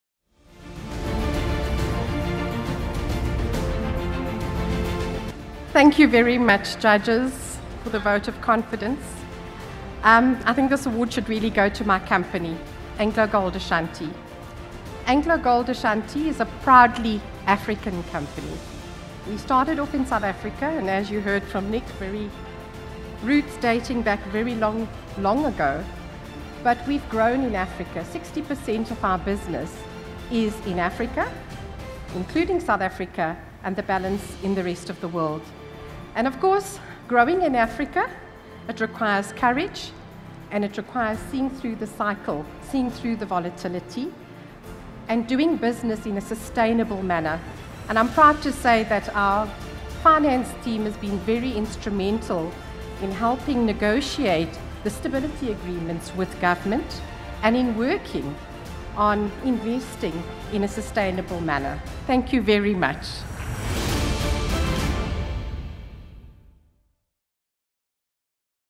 2018 CFO Awards Christine Ramon wins Moving into Africa Award
It is the first time in the five-year history of the event that a finance executive went home with no less than four prizes, with Christine also being honoured with the Compliance & Governance Award, the High Performance Team Award and the Moving into Africa Award. During her interview with the judges, Christine explained that her finance team is helping the company to move down the cost curve by being a true business partner, while progressing value-adding opportunities. She also told the judges that role modelling, being open and transparent, and communicating with people are among her best leadership traits, adding that she likes to give people accountability and empower them, but likewise expects them to keep her informed.

Book your seat for the Oscars for finance on 9 May 2019.